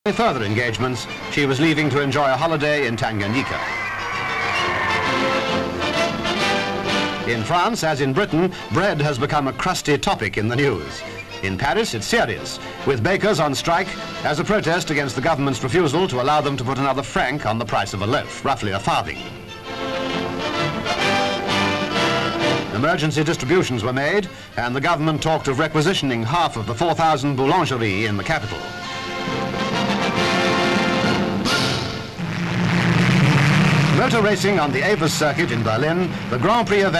BAKERS STRIKE IN PARIS
(24 Sep 1956) In Paris, the Bakers were on strike, as a protest against the Government's refusal to allow them to put another franc on the price of a loaf.